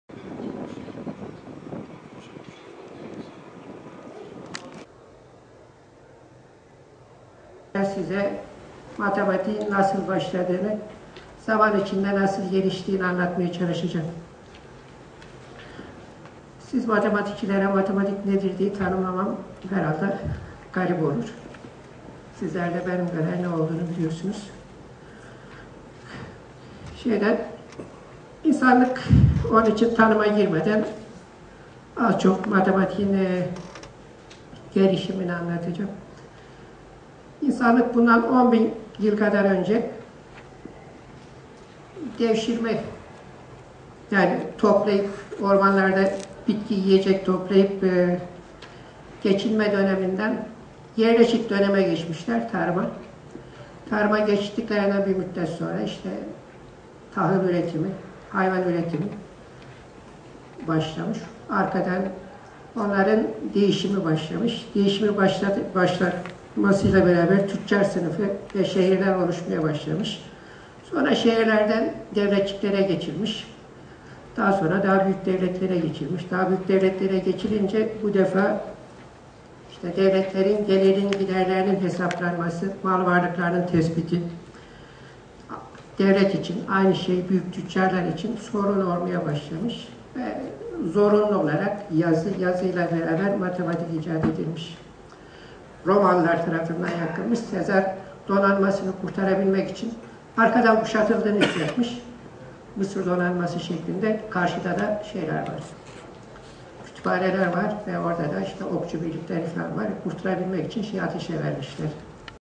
Matematikçiler Diyarbakır'da buluştu - 04 Eylül 2013
Türkiye'nin çeşitli üniversitelerinden çok sayıda bilim insanın katıldığı "26. Ulusal Matematik Sempozyumu" Dicle Üniversitesi'nde gerçekleştirildi.

Değerli İzleyicilerimiz;

TÜRKÇE

KÜRTÇE-KURMANCI

ARAPÇA

İNGİLİZCE

FARSÇA

KÜRTÇE-SORANİ